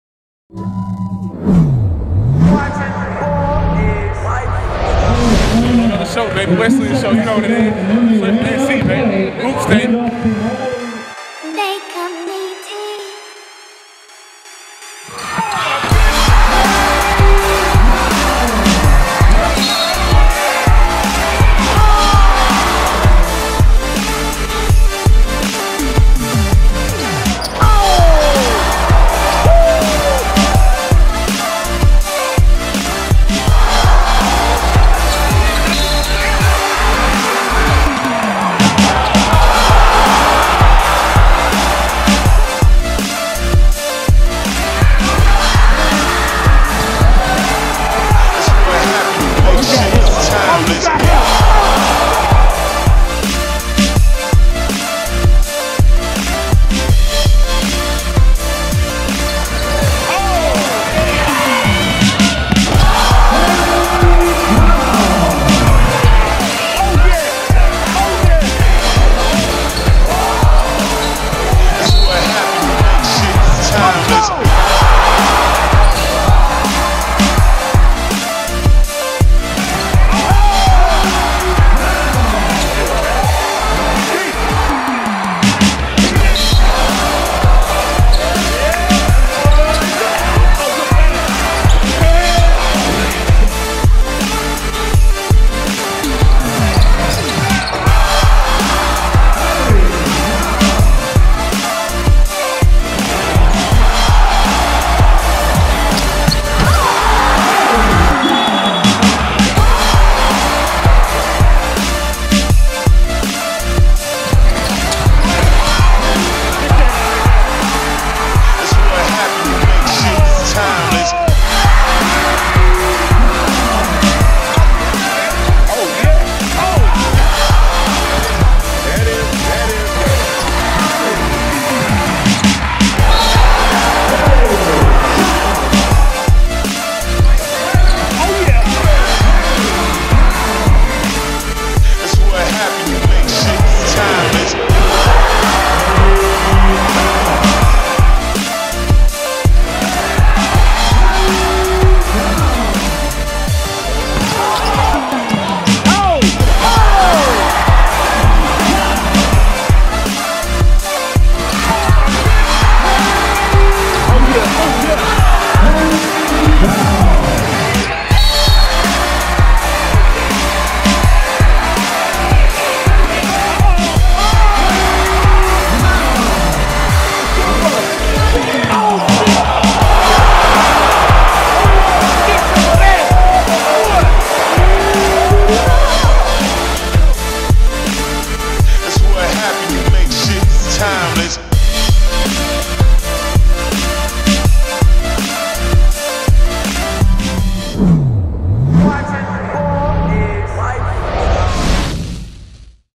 16 Y/O Harry Giles is the Best NBA Prospect in High School Basketball
After sitting out his sophomore season due to a knee injury, Harry Giles III entered the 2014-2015 high school basketball season hungrier than ever. Giles dominated opponents on a nightly basis, easily averaging a double-double. HGIII is considered the best junior in the country and many consider him to be the best NBA draft prospect, regardless of class. The Trojans lost in the state championship game to inner-county rival, Greensboro Day, and Giles is looking forward to taking out that aggression this summer on the circuit. To keep track of HGIII and everything HoopState keep it locked to the channels below!